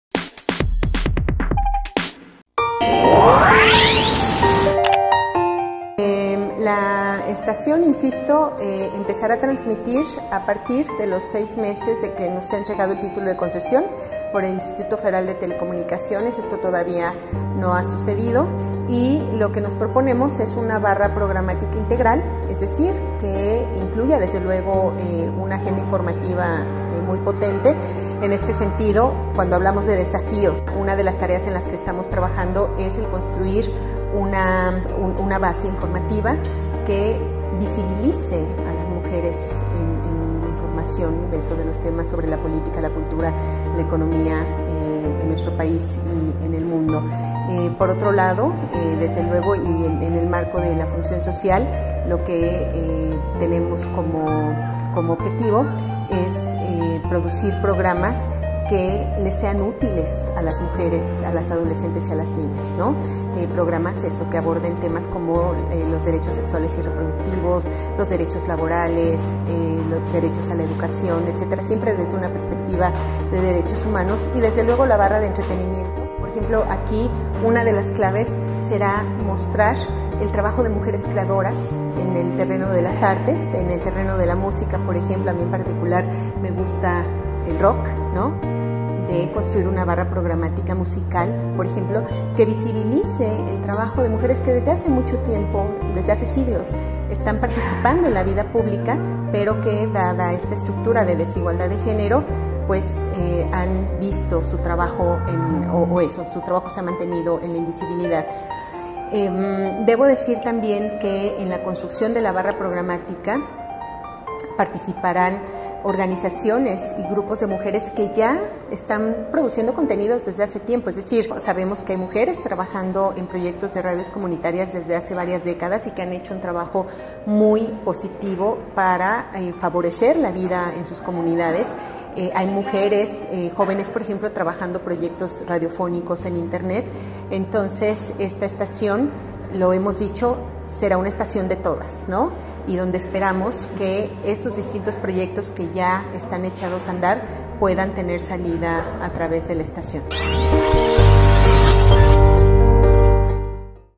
Radio Violeta, primera radiodifusora feminista en México
La estación empezará a transmitir el próximo año, tendrá una barra programática de noticias y otra de entretenimiento